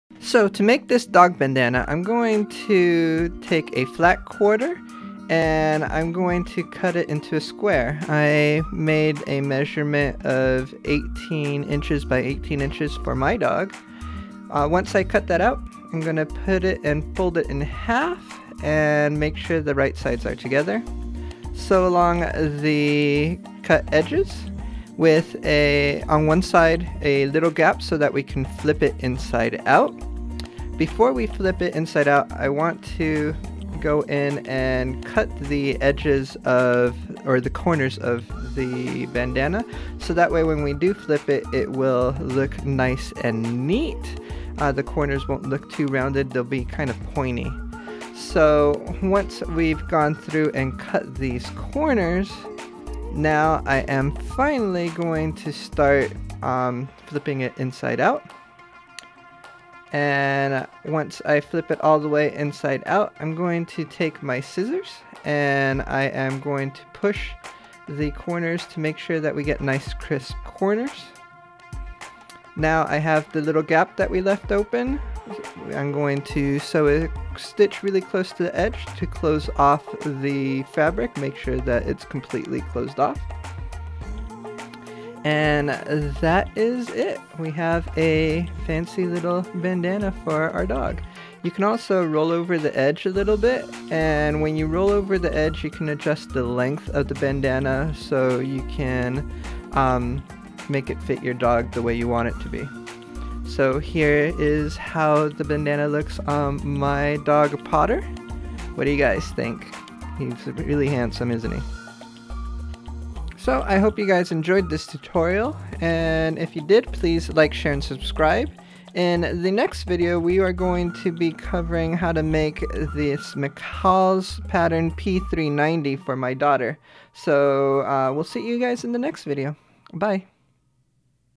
DIY Sewing Tutorial: How to Sew a Stylish Dog Bandana
In this DIY sewing tutorial, I go over how to sew a stylish DIY bandana for my dog Potter. This dog bandana is a quick simple project that is suitable for beginners. The Dog bandana takes about 10 - 15 minutes to finish. I hope you enjoy this do it yourself sewing project that will expand your dog's fashion wardrobe.

For this Sewing project you will need:
- Fat quarter of fabric cut down to 18"x18" or another square size suitable for the size of your dog.
- Thread
- Sewing Machine
- Sewing Shears

Fat Quarter Bundle
Next Pattern McCall’s Pattern P390
Sewing Machine I use
Sewing Shears I use

Recorded on Sony A65
Sony Alpha 16mm - 50mm Lens